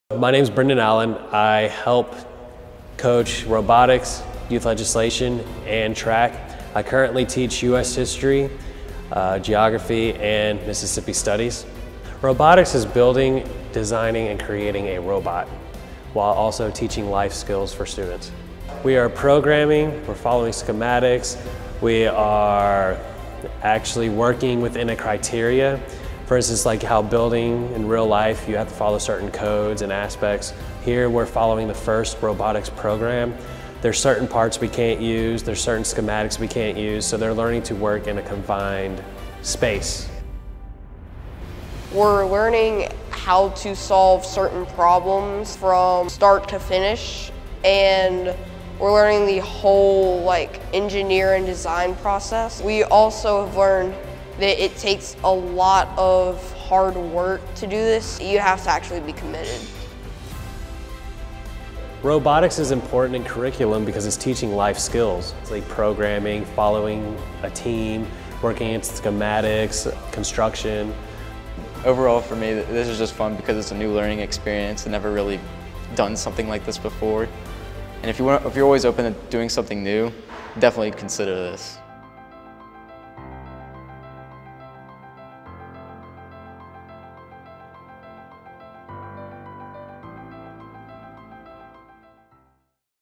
Saint Stanislaus Catholic High School Robotics Program
Robots at the junior high and high school level are both fun and highly educational.  Our robotics  programs covers many disciplines including mechanical design / assembly, electronics and programming.

LEARN MORE ABOUT SAINT STANISLAUS - THE #1 CATHOLIC SCHOOL AND BOARDING SCHOOL IN MISSISSIPPI ACCORDING TO NICHE.